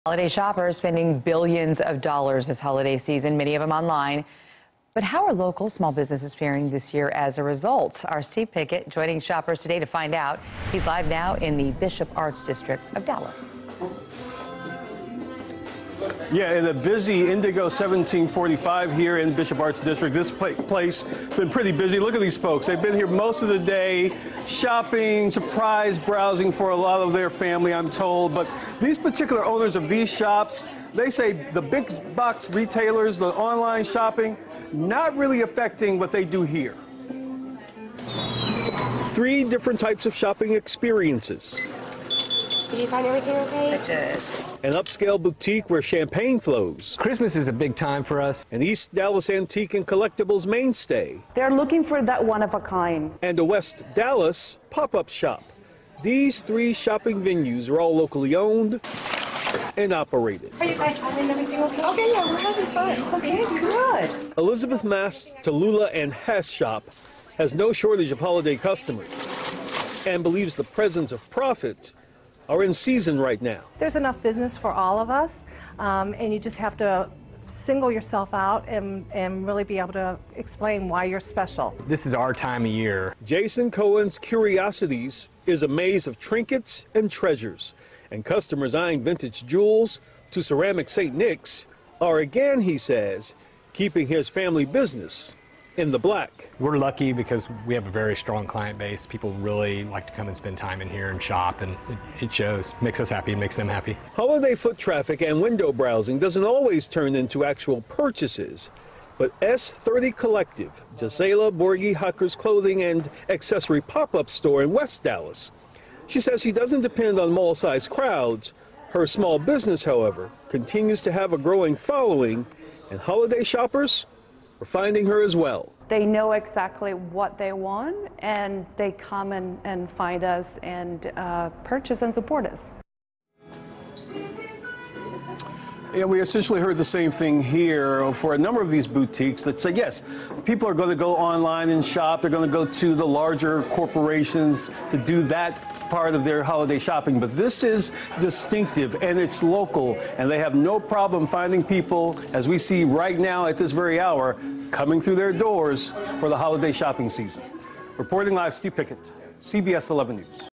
Dallas Area Small Businesses Say They Are Doing Well
Small business owners tell CBS11 the big box stores and online shopping isn't hurting them.